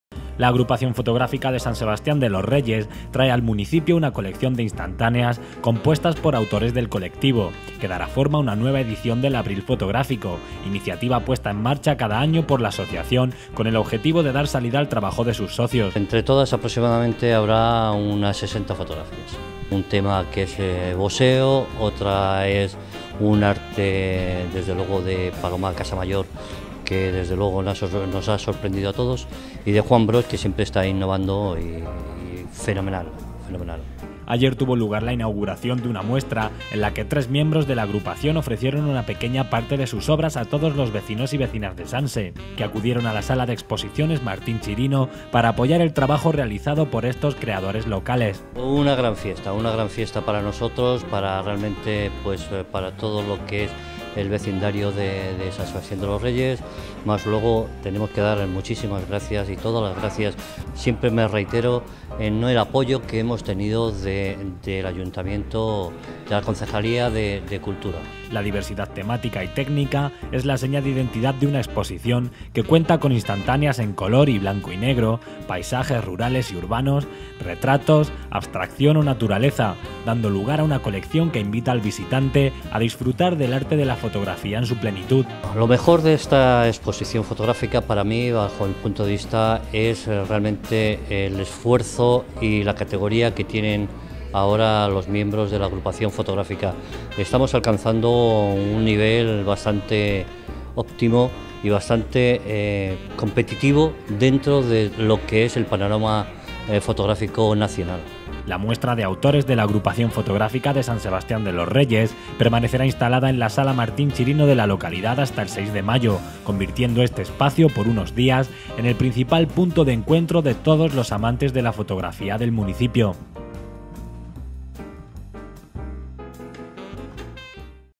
La AFSSR trae a Sanse su exposición de autores dentro del Abril Fotográfico
José A. MARTÍNEZ / San Sebastián de los Reyes, Madrid / 23.04.14
La Agrupación Fotográfica de San Sebastián de los Reyes (AFSSR) trae al municipio una exposición compuesta por obras de tres autores miembros del colectivo. La muestra está incluida dentro de la iniciativa "Abril Fotográfico", puesta en marcha cada año por la AFSSR, que pretende crear un espacio donde mostrar el trabajo de sus socios a la par que divulga y promociona el arte de la fotografía. Del paisaje al retrato; del color al blanco y negro. La selección ofrece al visitante un amplio abanico de técnicas y estilos.
La exposición de autores de la AFSSR permancerá instalada en la sala de exposiciones Martín Chirino hasta el 6 de mayo.